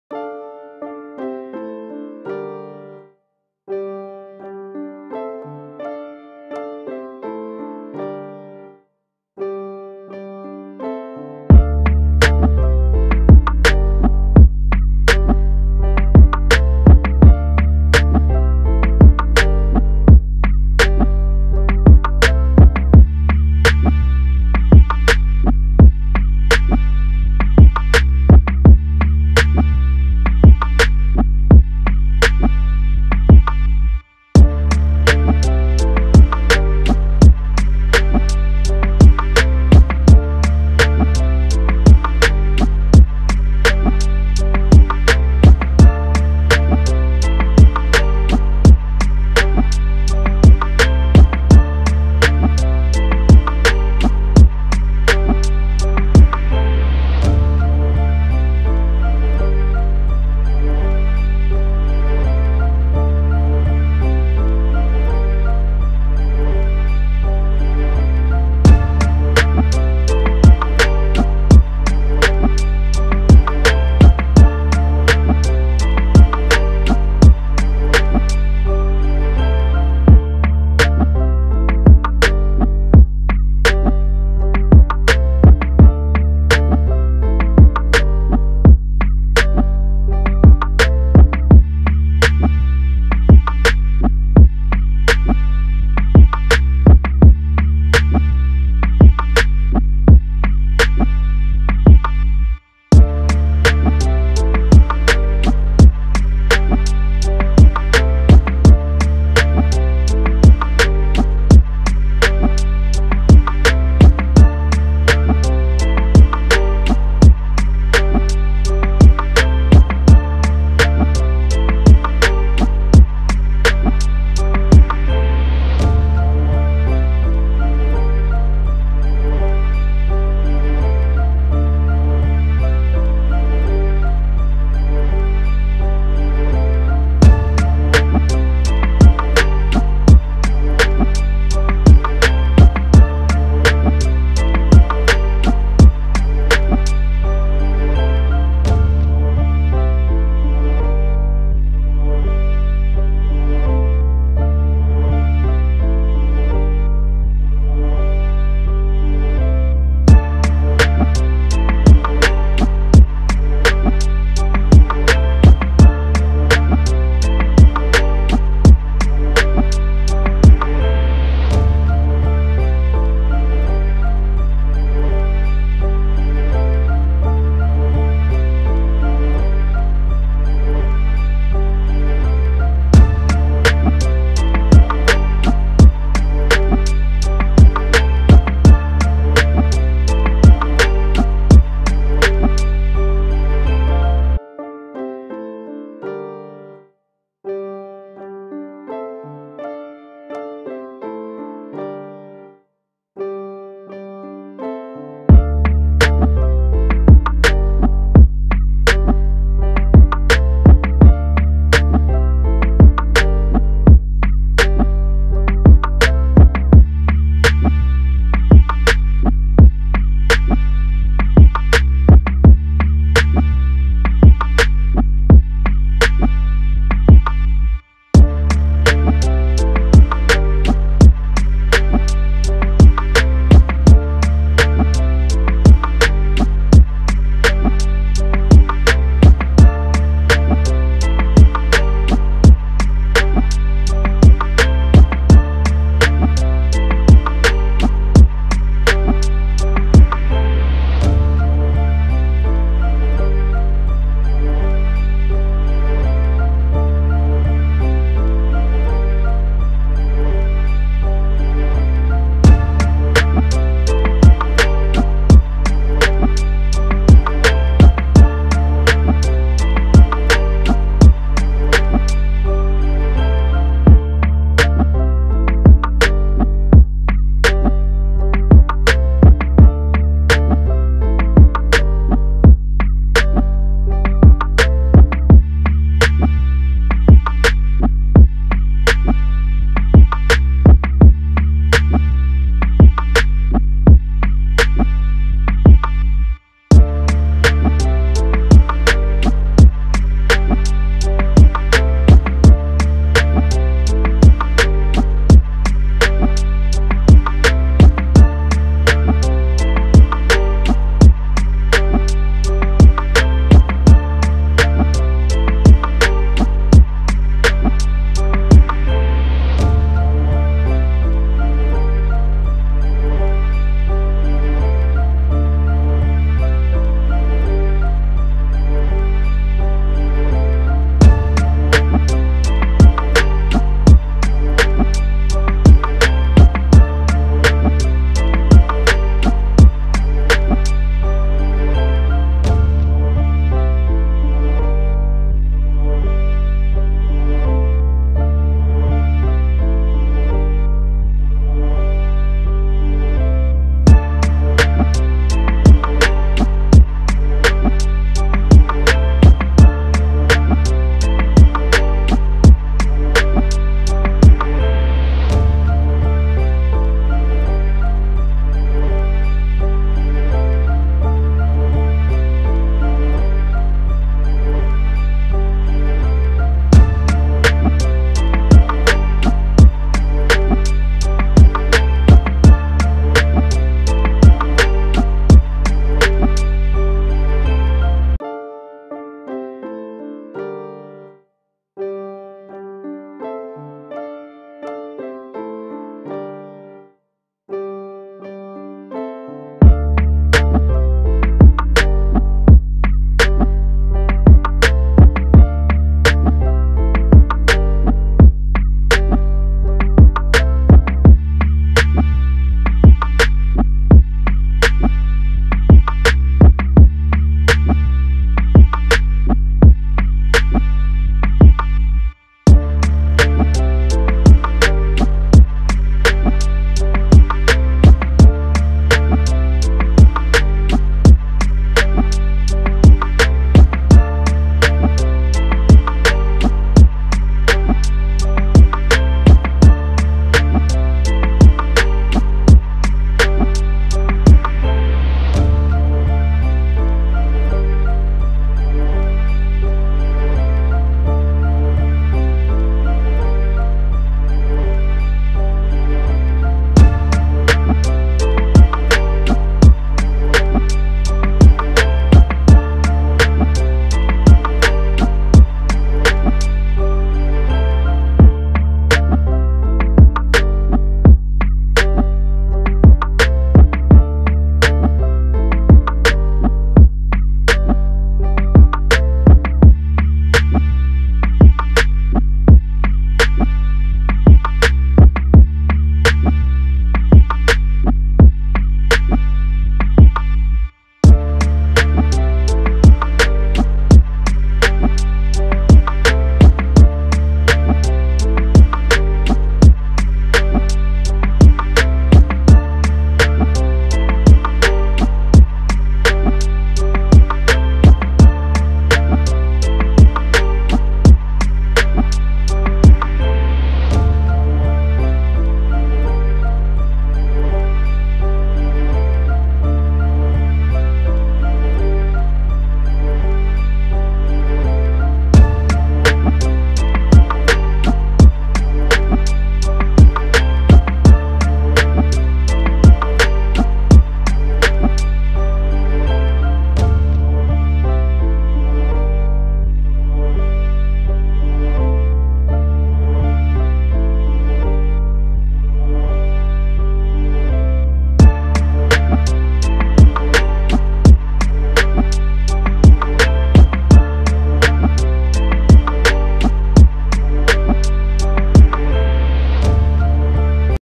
Review menu giá đồ ăn nước uống tại quán Mì Sukiya Nhật Bản | Tạ Quang Bửu Quận 8 TP.HCM
Review manu giá đồ ăn nước uống tại quán Mì Sukiya Tạ Quang Bửu Quận 8 TP.HCM
mi sukiya